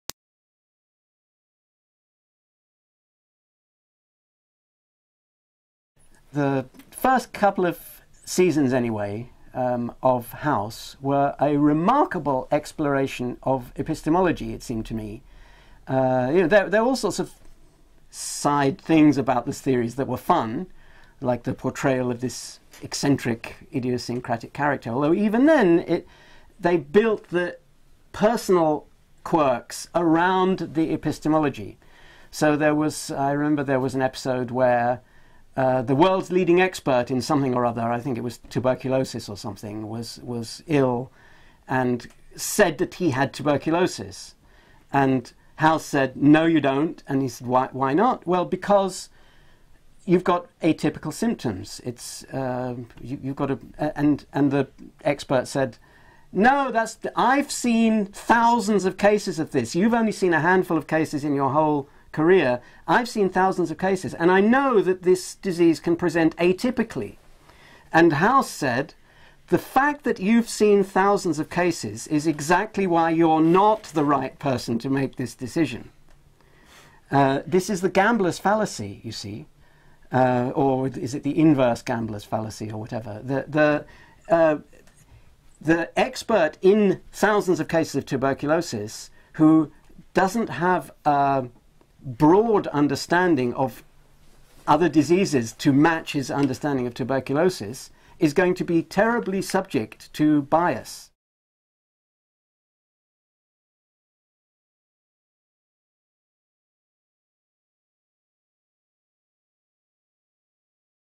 The Philosophy of House
David Deutsch says that the TV show illustrates one important way our thinking goes astray.